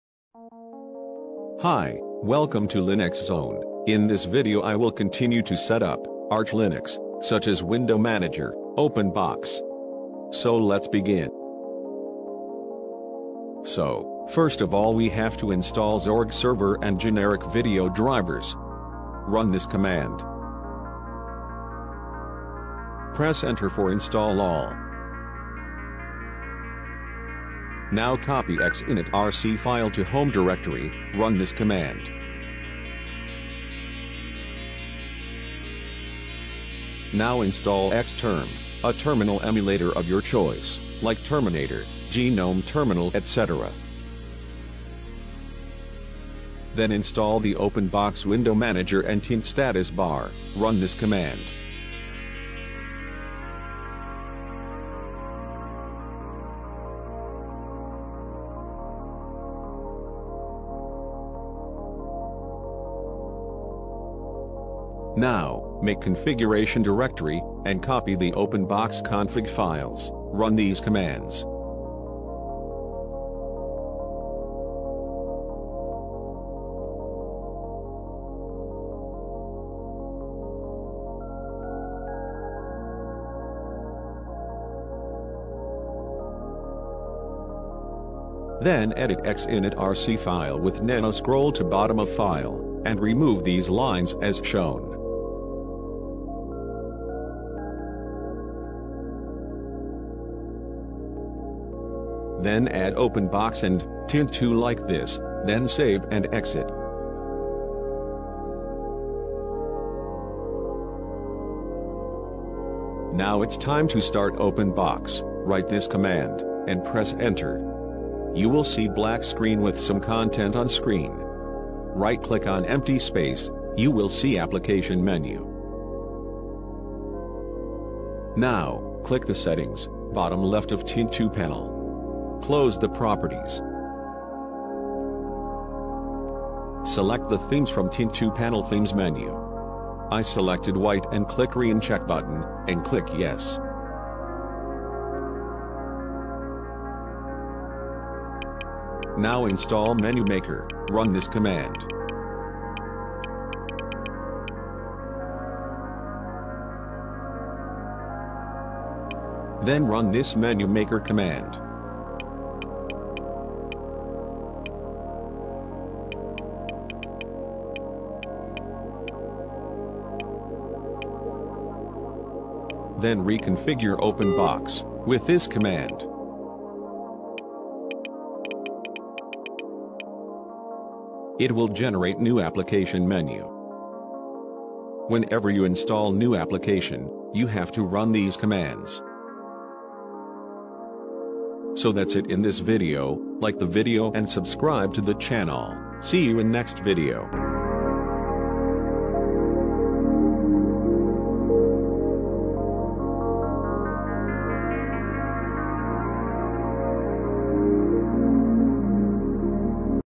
Arch Linux install (OpenBox window manager) Part-3
Arch Linux install OpenBox window manager installation part 3,
Hi, this video is continuation of Arch Linux Installation EFI 2022, in this video I will show how to install Open Box window manger, with xinitrc, no display manager ok.